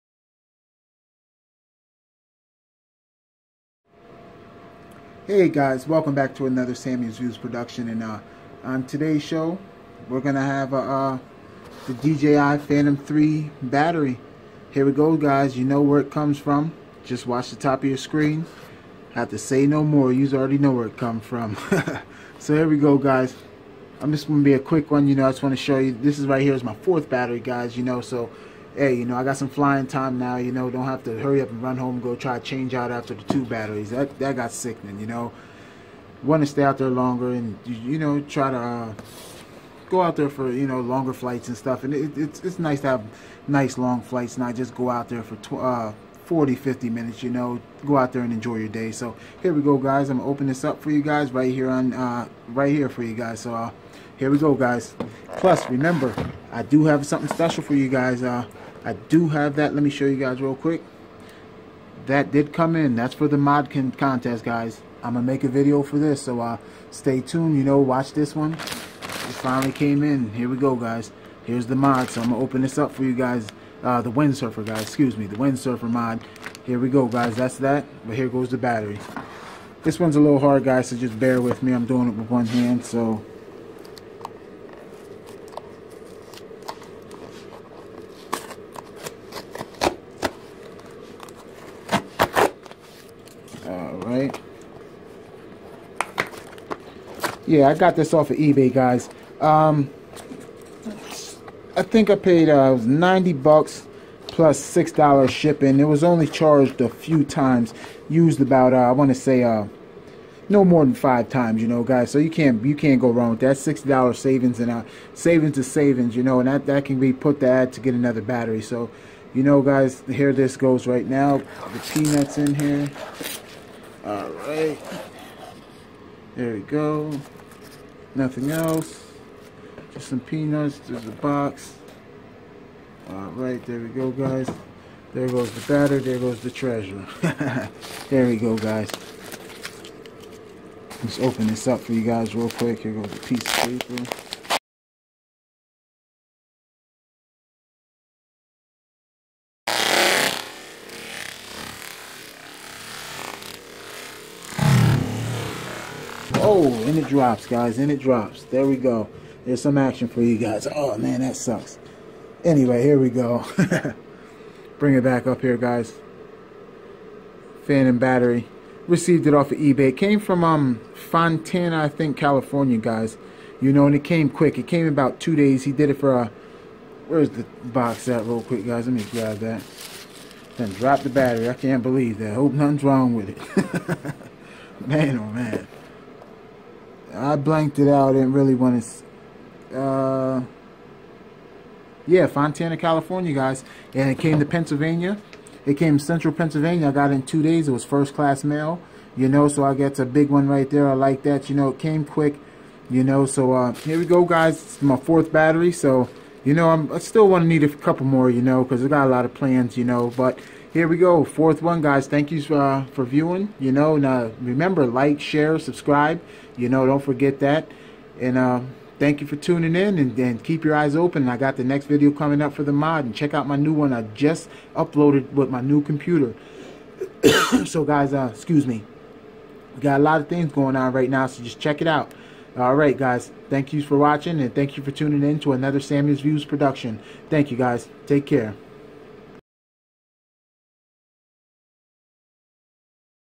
DJI Phantom 3 Standard - Battery Review and VIDEO BLOOPER
Here is my 4th battery that i just received. Edited with new computer. I Hope you guys like!!

ALOT MORE COMING GUYS, WATCH OUT FOR IT!!!